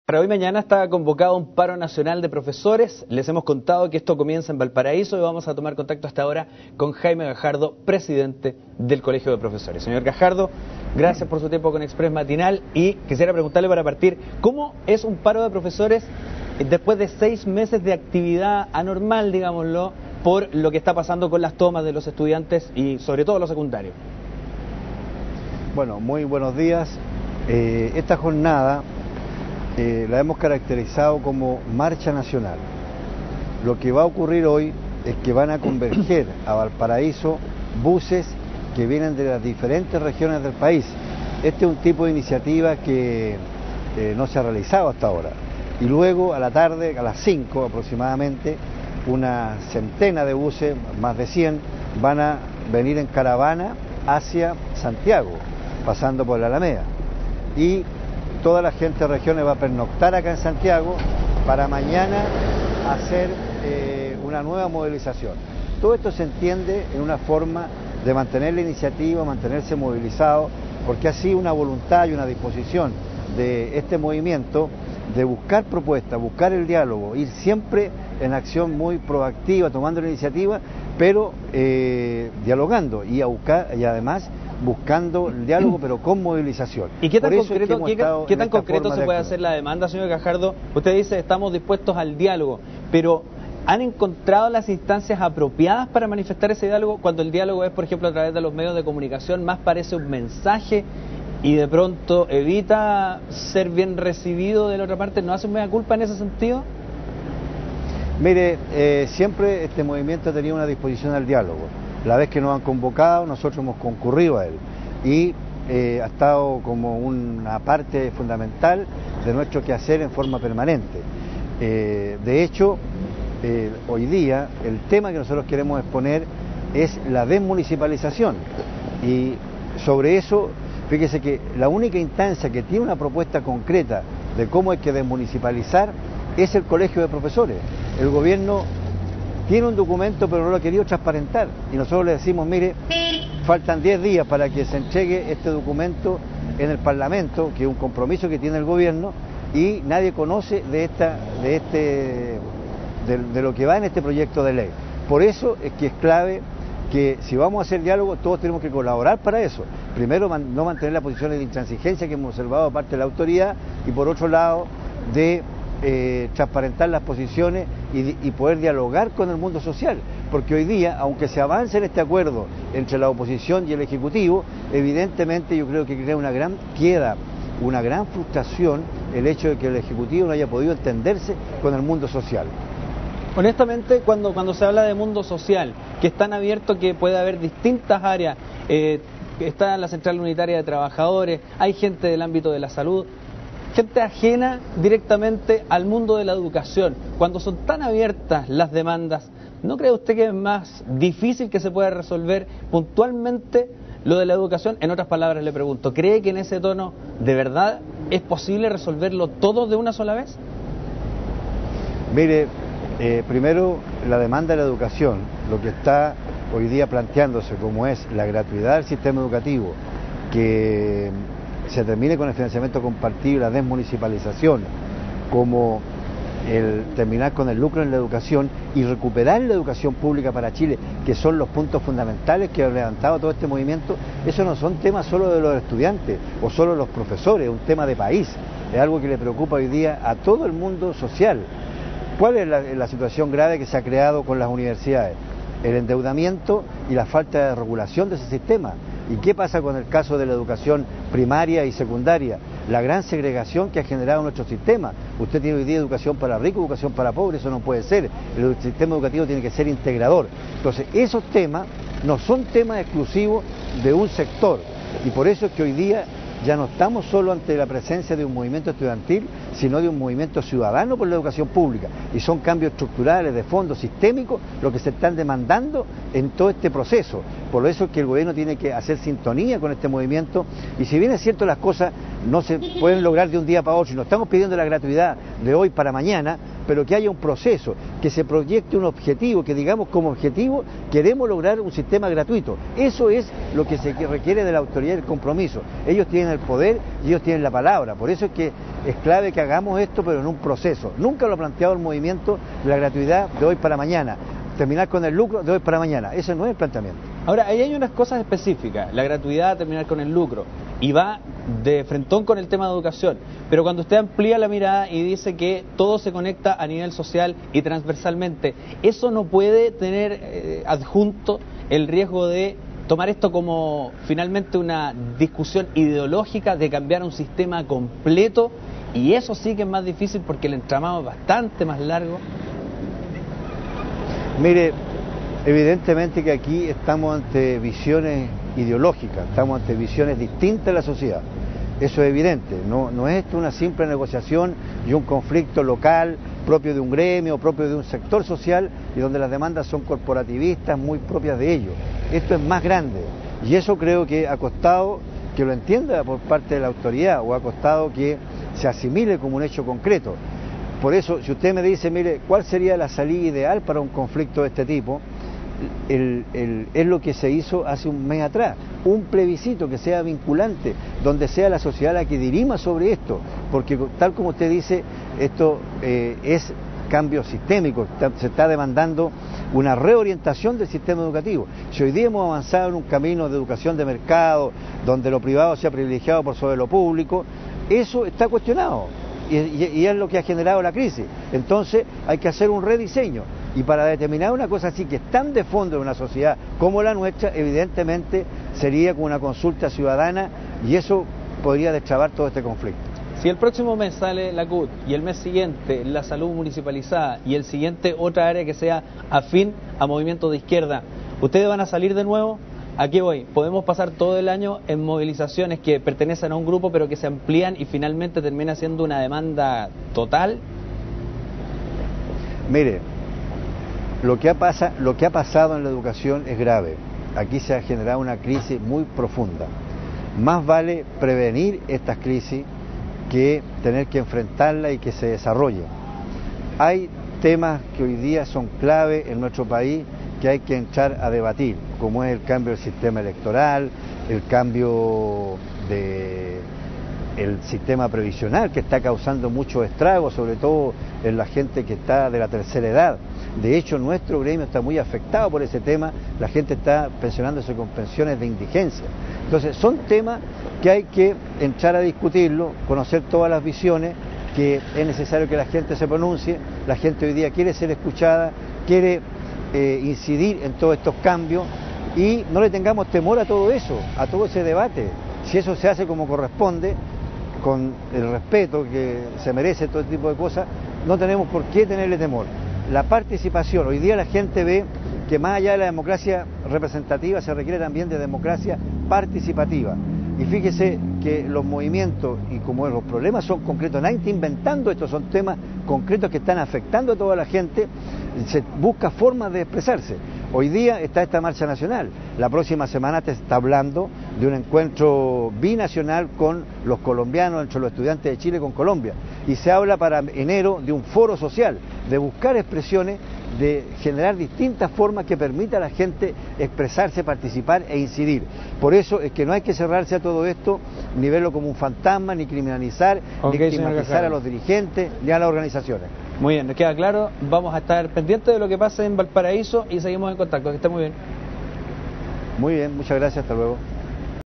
Entrevista a Jaime Gajardo, presidente del Colegio de Profesores
"Este movimiento siempre ha tenido voluntad al diálogo", dijo respecto al paro nacional de 48 horas que comienza este jueves.